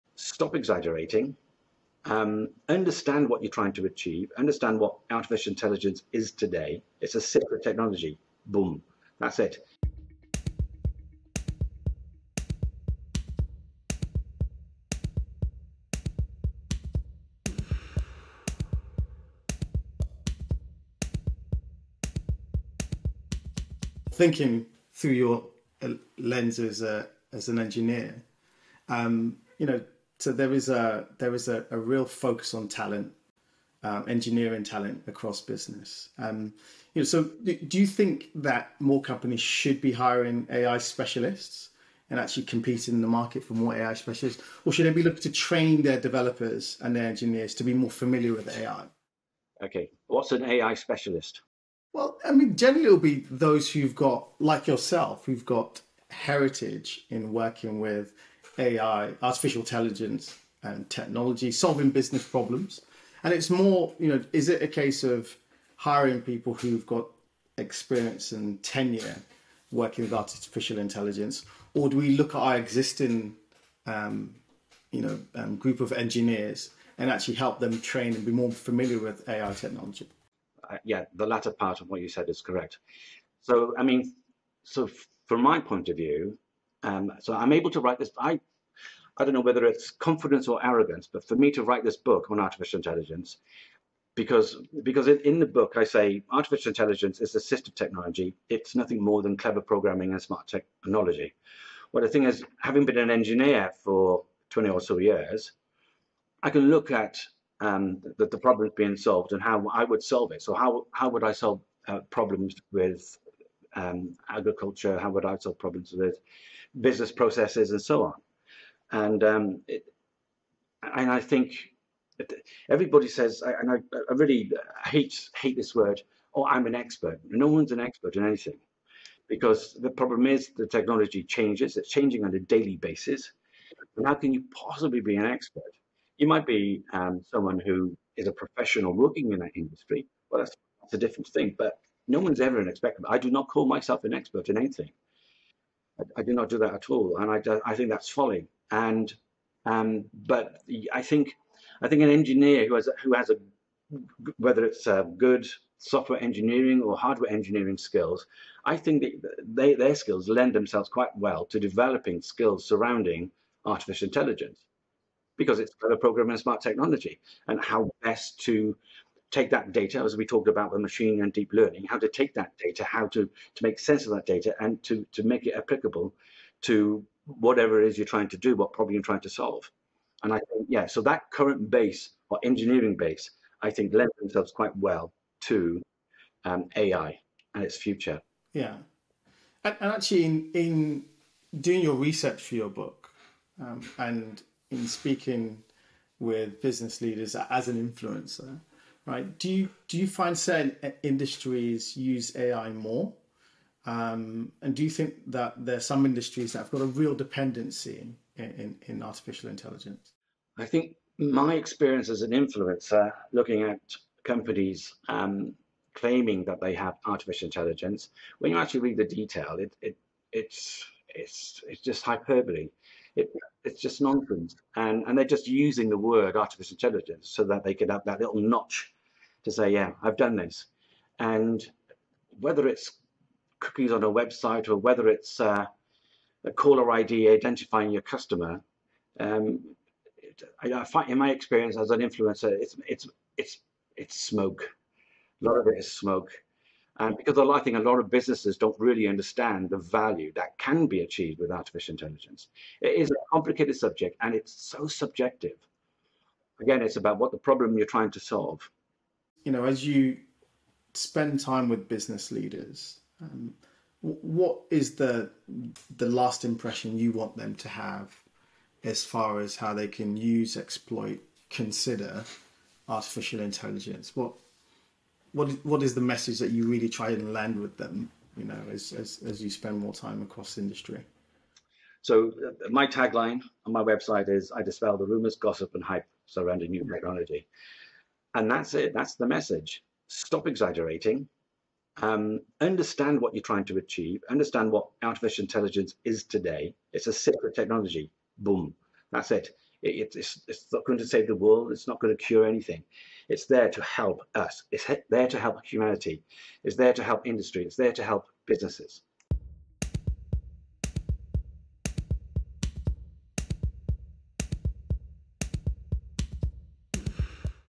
Engineering talent and the myth of AI expertise
Where we can find more AI talent? Uche talks with Dean Gratton, tech futurist and author, about skills and the myth of AI expertise. And Dean shares his views on the widespread AI hyperbole that exists at the moment.